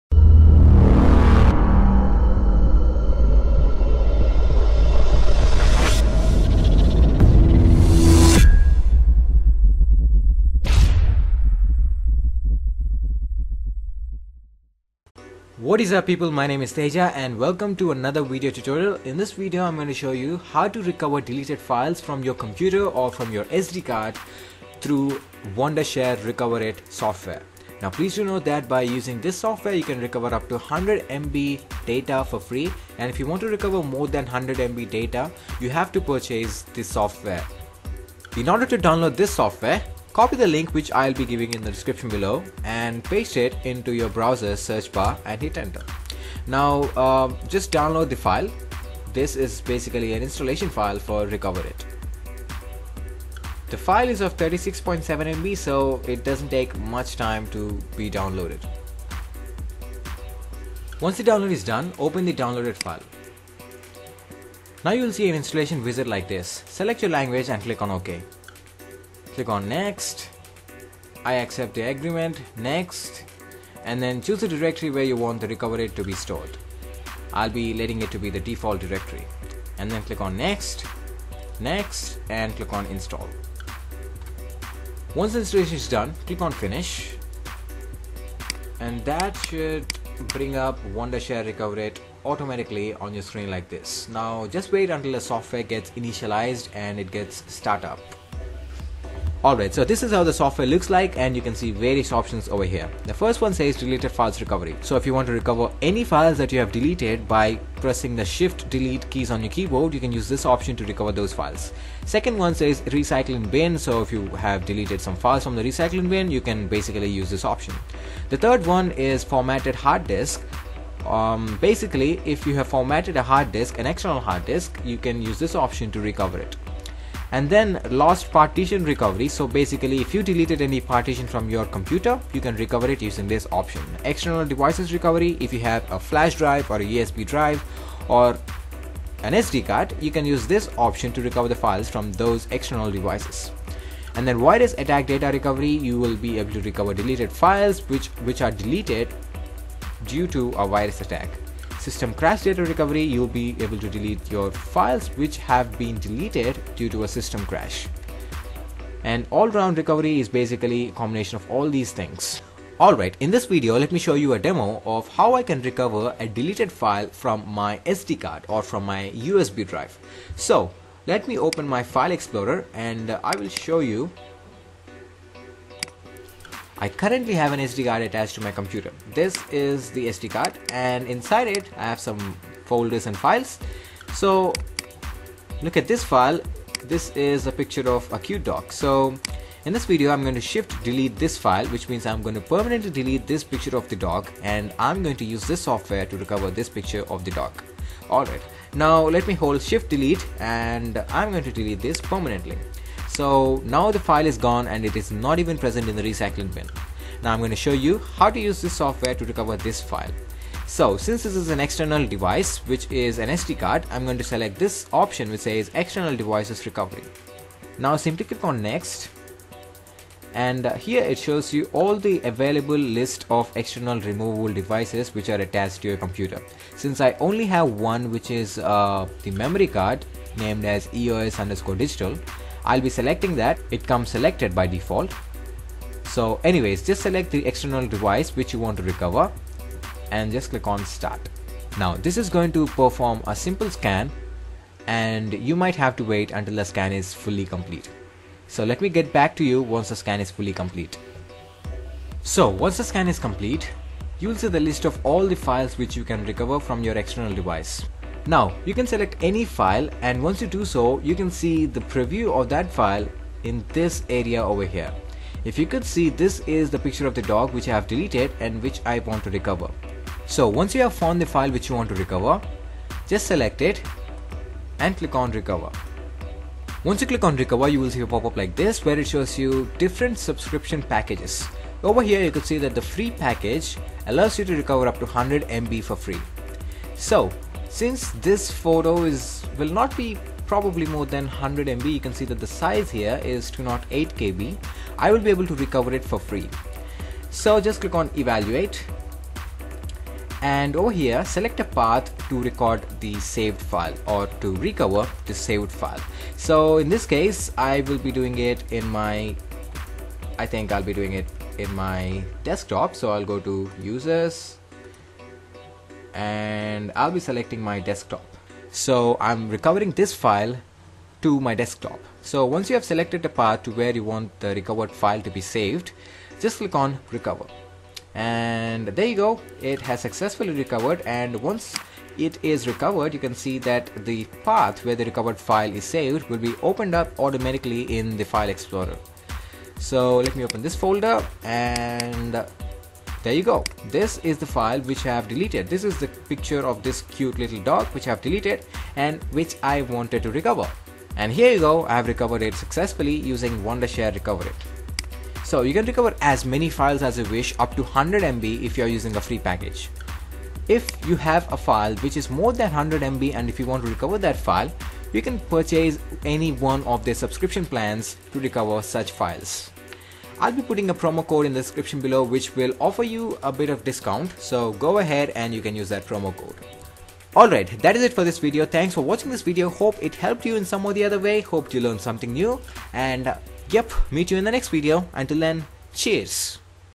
Recover Deleted Files from your Computer/USB/MemoryCard
In this video, I will teach you how to recover your deleted files using Wondershare RecoverIt.

If you like this video, don’t forget to leave a thumbs up below. Cheers!